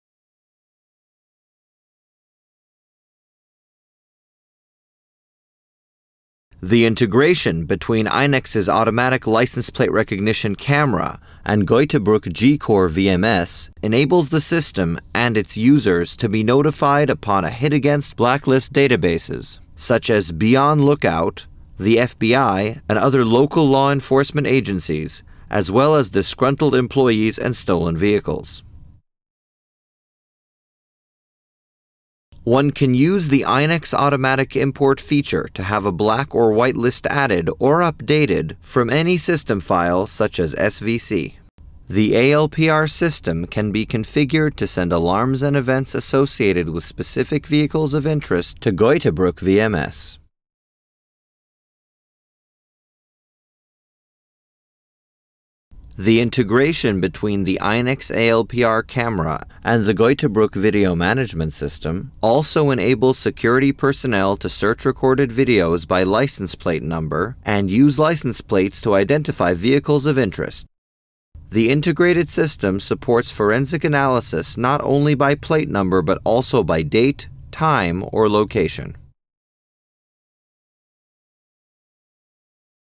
GEUTEBRUCK VMS and INEX ALPR INTEGRATION
The integration between INEX TECHNOLOGIES’ ALPR (Automatic License Plate Recognition) cameras and Geutebruck VMS enables the system to search recorded video by license plate, and use license plates to identify vehicles of interest